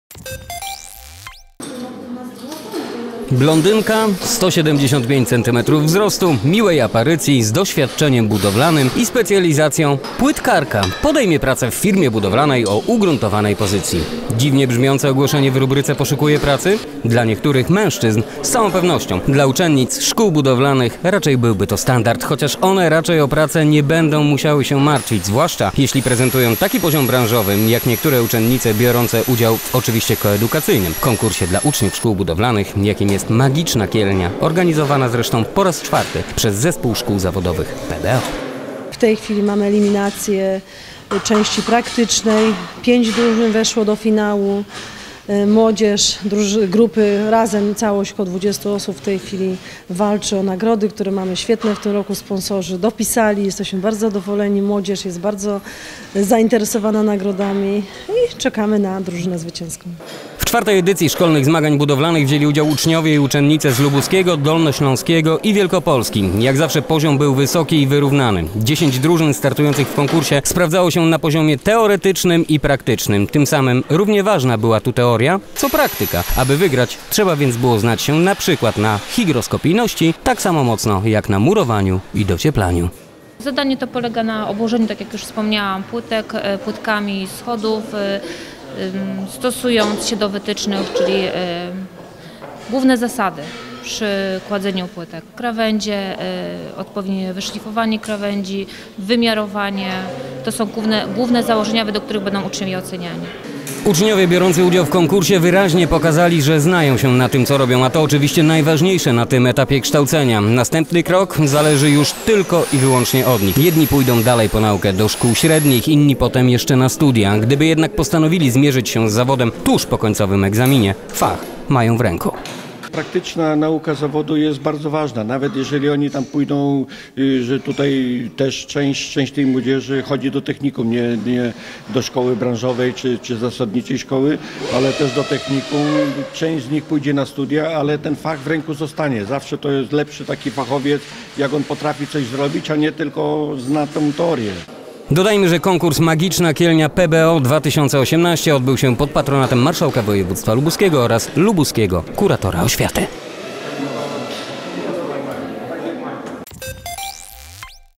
TvZG.pl - Magiczny fach w rękach
Blondynka, 175 centymetrów wzrostu, miłej aparycji z doświadczeniem budowlanym i specjalizacją płytkarka, podejmie pracę w firmie budowlanej o ugruntowanej pozycji. Dziwnie brzmiące ogłoszenie w rubryce poszukuję pracy? Dla niektórych mężczyzn z całą pewnością. Dla uczennic szkół budowlanych raczej byłby to standard. Chociaż one raczej o pracę nie będą musiały się martwić, zwłaszcza jeśli prezentują taki poziom branżowy, jak niektóre uczennice biorące udział w - oczywiście koedukacyjnym - konkursie dla uczniów szkół budowlanych, jakim jest Magiczna Kielnia organizowana po raz kolejny przez Zespół Szkół Zawodowych PBO.